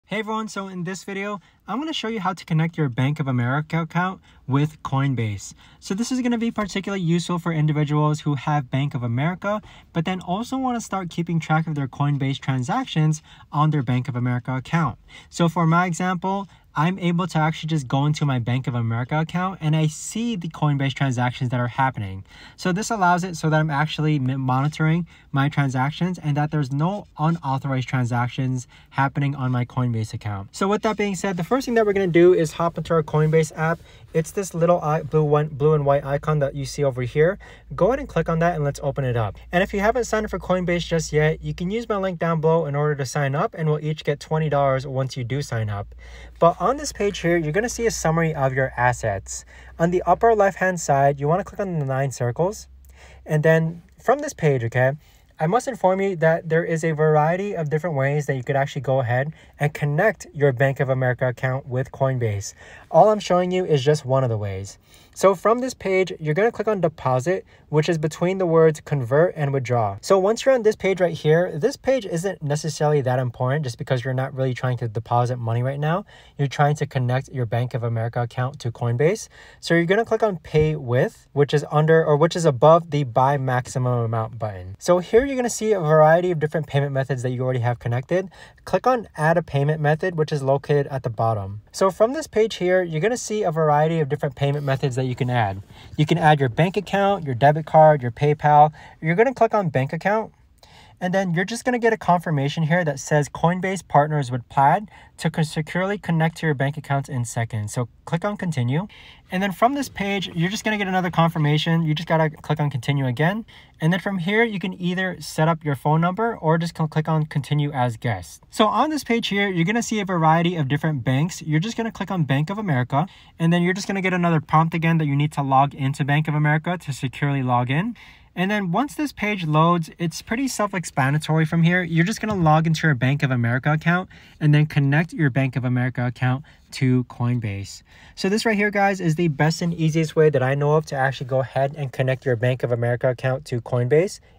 How To Connect Bank Of America Account With Coinbase (SIMPLE & Easy Guide!)
Looking to link your Bank of America account with Coinbase? This step-by-step guide makes it simple and hassle-free! In this video, we’ll show you how to securely connect your bank account to Coinbase for seamless deposits and withdrawals. Learn how to navigate the Coinbase app, verify your bank details, and ensure a smooth setup process. Stay in control of your crypto transactions with this straightforward tutorial.